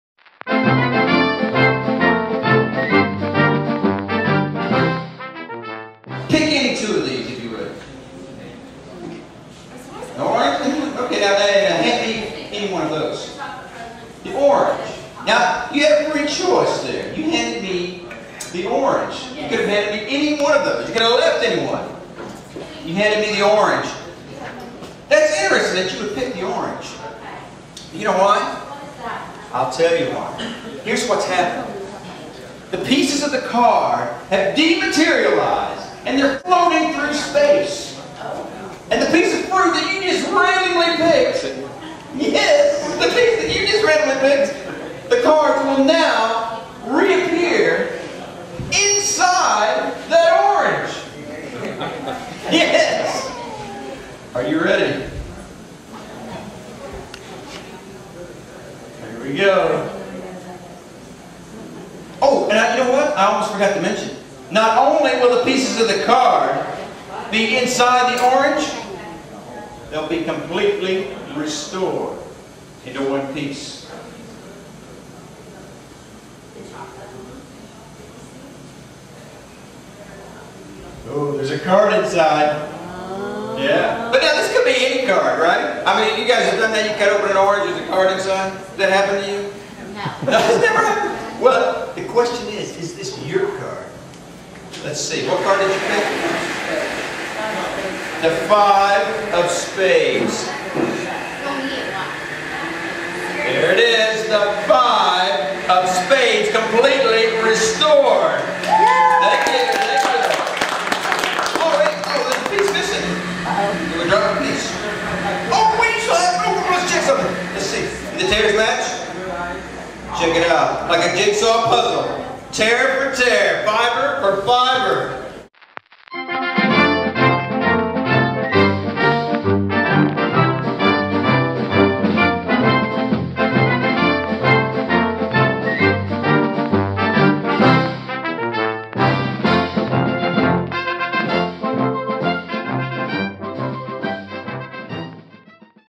The Amazing Zane Performs the Fruitful Card.m4v
Illusionist Zane Harris (The Amazing Zane) Performs the Fruitful Card